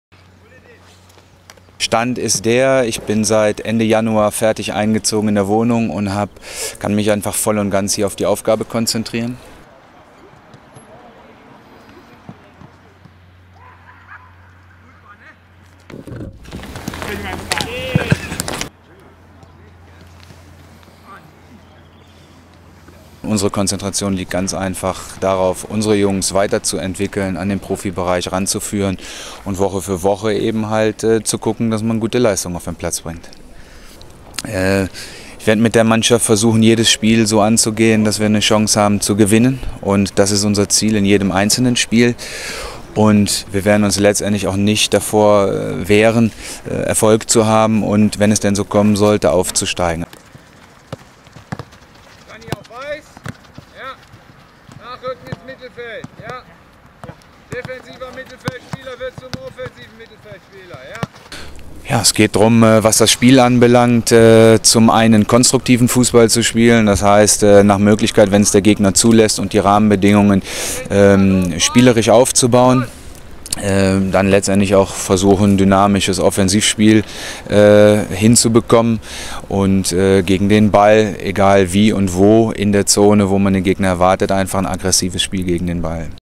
Interview Markus von Ahlen (U21-Fußball-Trainer, TSV 1860 München)
Gespräch mit Markus von Ahlen, Trainer beim TSV 1860 München, vor seinem ersten Punktspiel mit der U21-Mannschaft.